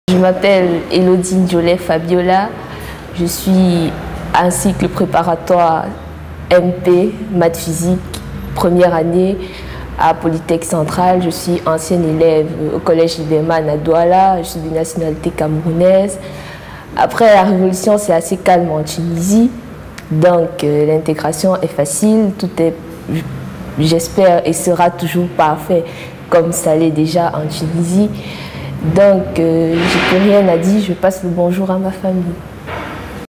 Ecole Polytechnique en Tunisie: polytechcentrale.tn
Cycle Préparatoire, cycle ingénieur,Licence, Master

Le Groupe Université Centrale est composé de quatre établissements d'enseignement supérieur et deux établissements de formation professionnelle, tous agréés par leurs ministères de tutelle.